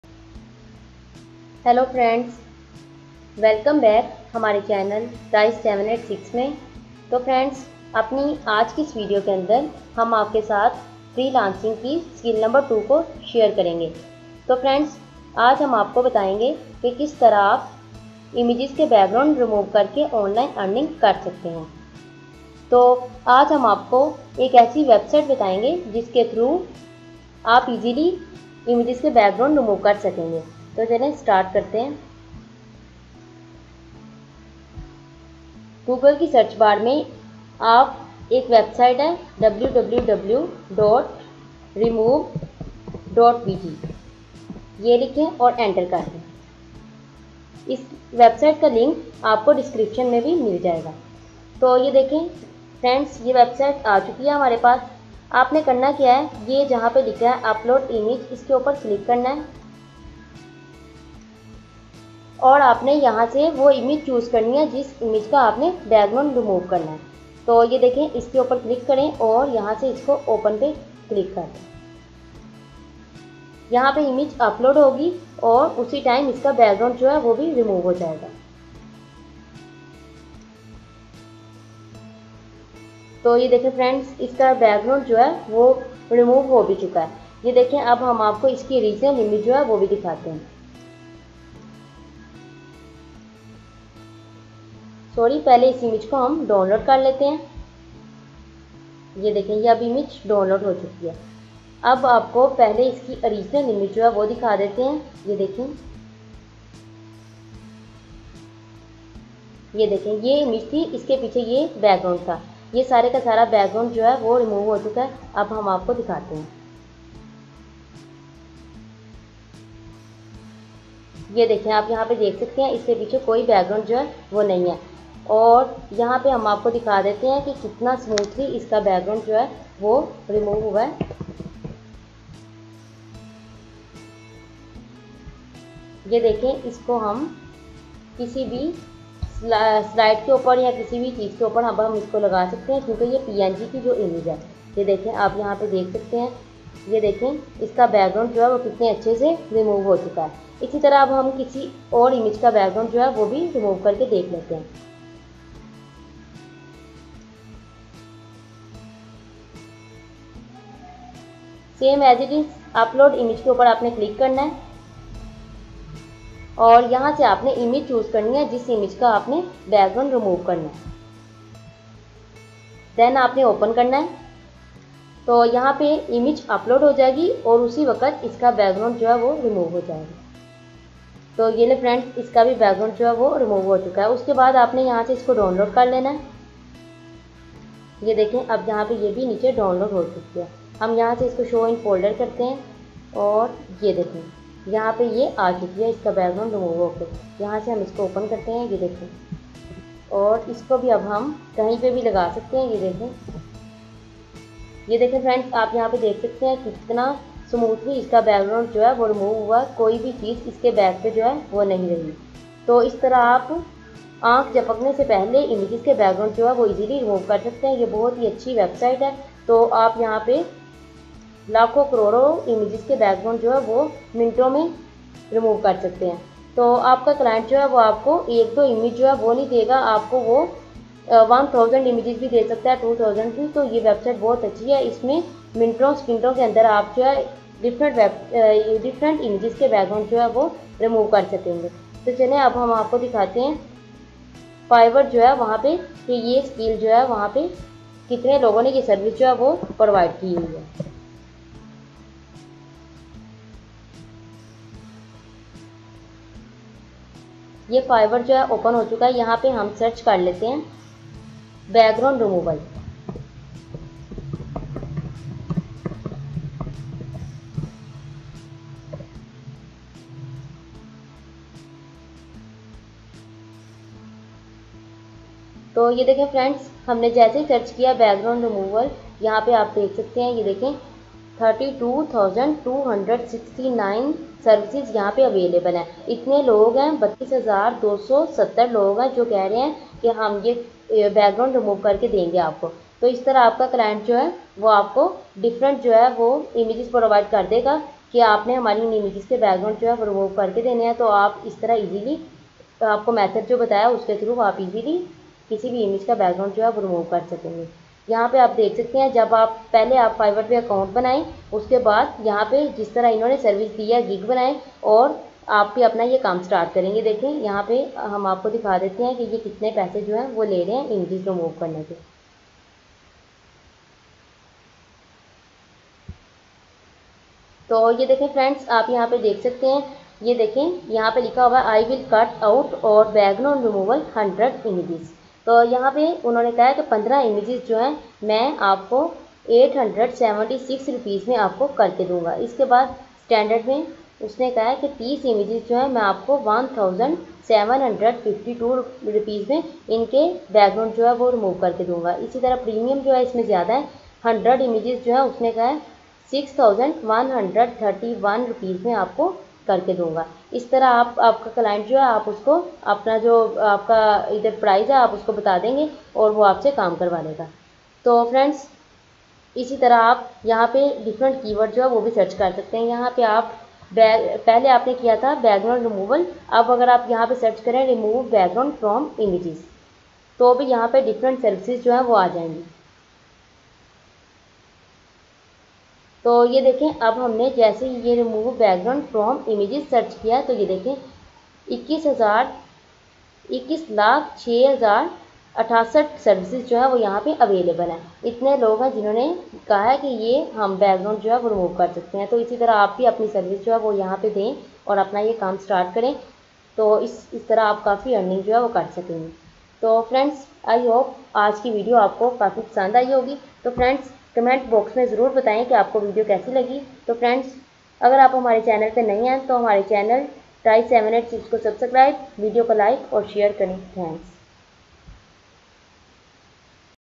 Skill # 2| Earn Money By Removing Background from Images | Ties 786
In this Tutorial we will discuss how to remove background from images and earn money online. I hope you will learn and gain knowledge from this tutorial. ||  ties 786